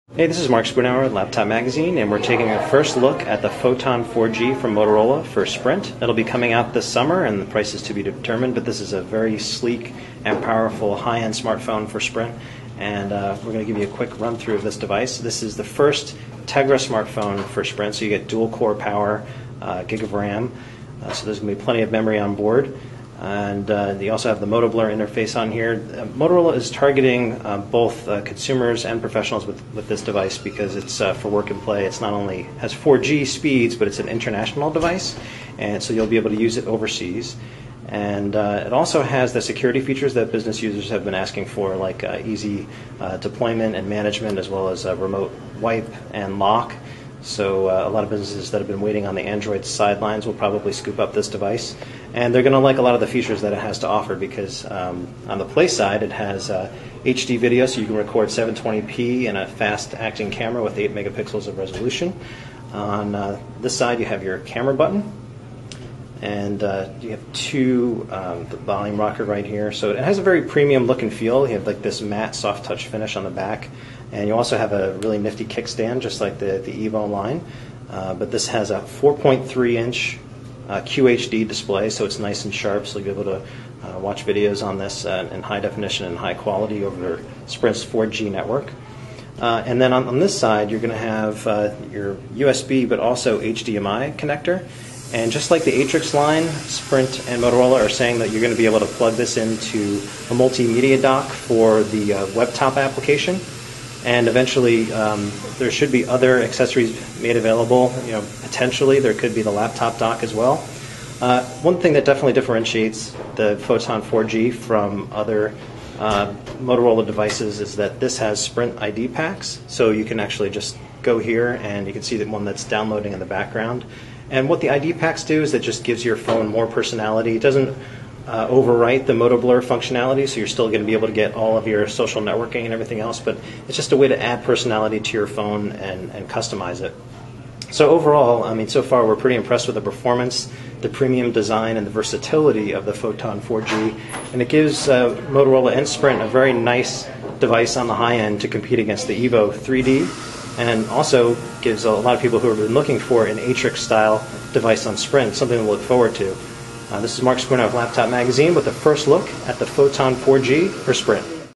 Motorola Photon 4G for Sprint Hands-on
LAPTOP Magazine takes a first look at the Motorola Photon 4G Android smartphone with Motoblur for Sprint, which features a dual-core Nvidia Tegra processor.